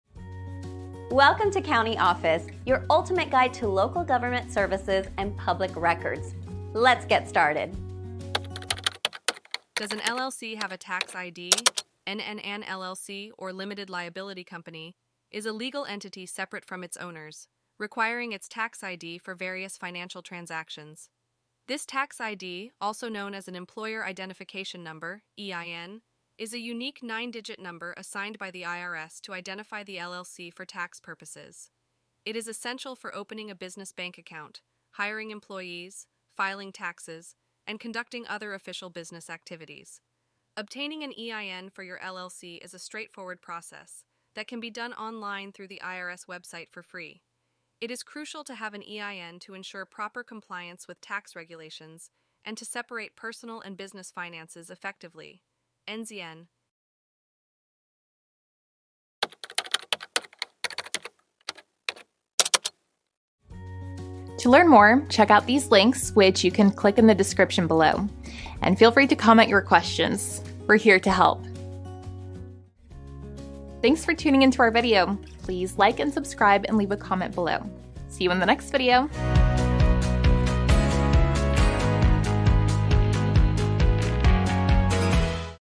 Does an LLC Have a Tax ID? - CountyOffice.org
Does an LLC Have a Tax ID? Have you ever wondered about the importance of having a Tax ID for your LLC? In this educational video, we delve into the significance of obtaining an Employer Identification Number (EIN) for your Limited Liability Company. An LLC is a separate legal entity that requires a Tax ID for various financial transactions and official business activities. The EIN, assigned by the IRS, is crucial for tasks such as opening a business bank account, hiring employees, filing taxes, and ensuring compliance with tax regulations. Obtaining an EIN for your LLC is a simple process that can be done online through the IRS website at no cost. By having an EIN, you can effectively separate personal and business finances while adhering to tax requirements.